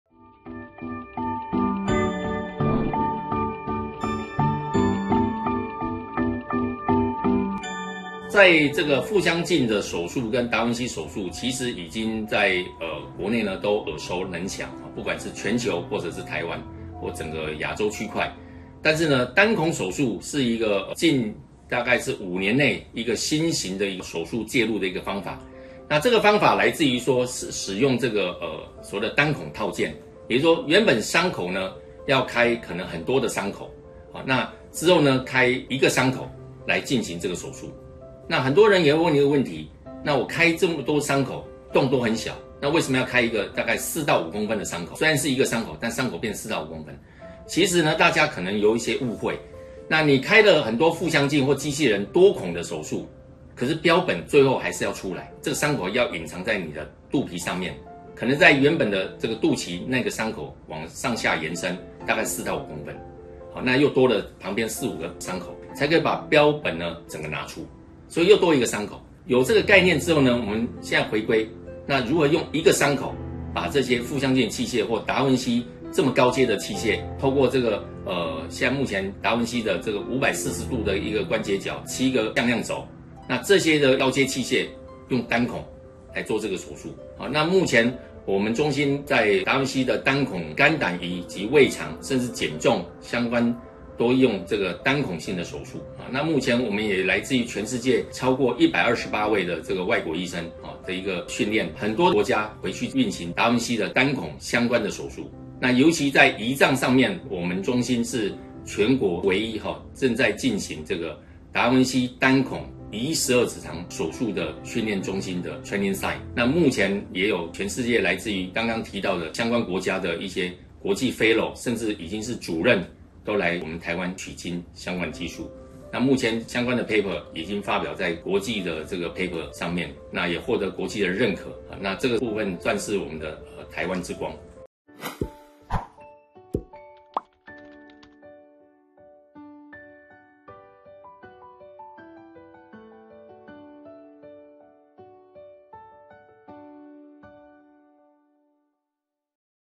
【胰臟癌達文西手臂單孔手術】彭正明醫師／中山醫學大學附設醫院｜手術善其事
►更多胰臟癌知識：【胰臟癌存活率高嗎？罹病原因、症狀、末期治療方式】

胰臟癌手術新進展：達文西手臂單孔手術

達文西機械手臂為國內著名的微創手術系統與技術，進入台灣大約15年的時間，逐漸為國人攻克比較艱難的手術任務，從鬼門關前救回許多人的寶貴性命。機械手臂的優勢在於超高解析的手術視野以及取代人手的560度高仿真手腕機械臂，傳統手術方式遭遇比較困難的手術，過程容易發生大量出血並且相當耗時、耗精力，透過機械手臂的幫助，外科醫師執行手術時能夠透過觀看螢幕為患者進行手術。機械手臂最大優勢就是低出血率，尤其精準定位在狹窄又血流量大的腹腔中活動自如，處理高難度的腹腔手術時更加得心應手。達文西手臂機械系統可以幫助醫師處理胰臟癌腫瘤時，利用機械手臂的七個關節進行周邊血管的剝離移除，避免傷害更重要的血管。

不過，因為達文西機械手臂手術是醫師利用遙控操作機械手臂進行手術，實務上會比真人的手多了一道隔閡，加上缺乏觸感在動刀路線的判斷上得要非常準確，因此達文西手臂也非外科手術的首選，而是得仰賴技術純熟的醫師與專業的醫療團隊，才能讓機械手臂發揮最大功效！若選擇新一代達文西機械手臂單孔手術，不會再像以往腹腔鏡或是既有達文西手術會開1個以上的2公分開口，這種新手術方式會改為開一個4-5公分的開口，而機械手臂會集中在這個開口進行作業，包含把切除下來的標本一同取出。

但是達文西機械手臂目前沒有健保給付，費用在新台幣20-35萬不等，除了有保險以外也並非一般家庭負擔得起，患者需審慎評估。